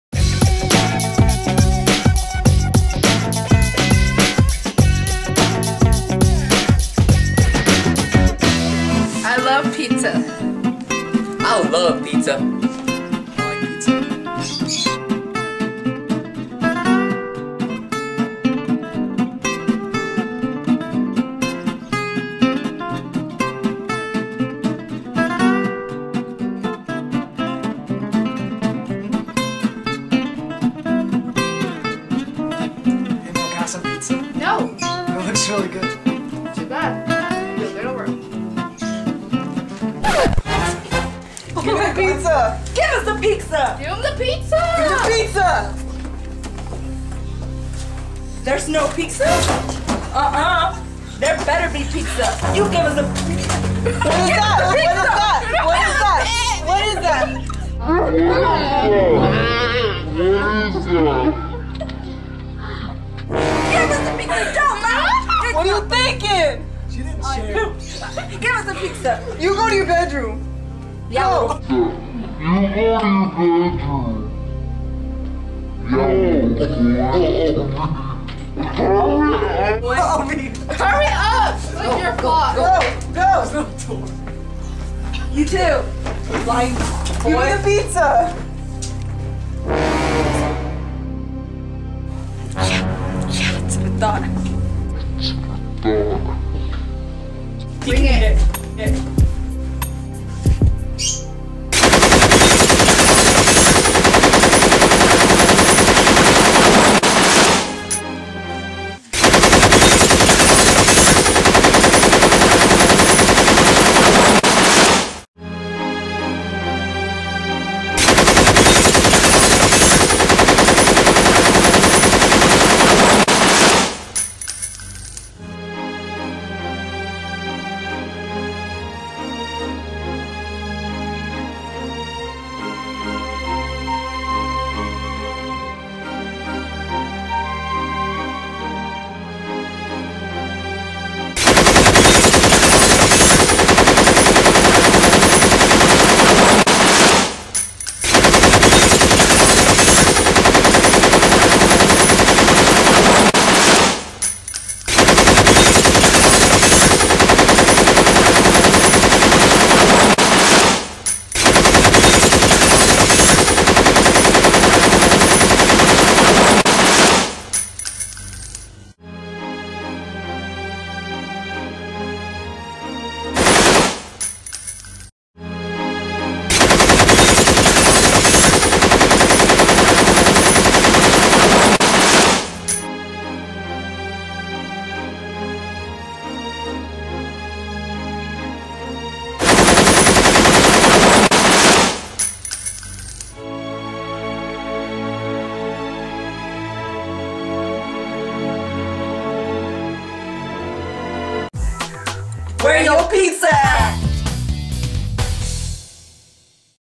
Pizza Hut Commercial with Guns
Pizza + Guns = Disaster when when a gang of two with guns barges into a happy families home and threatens to kill them unless they get some pizza NOW!!!
This commercial stars:
Brenda
Miguel
Peter
Yenifer
and guest star Taryn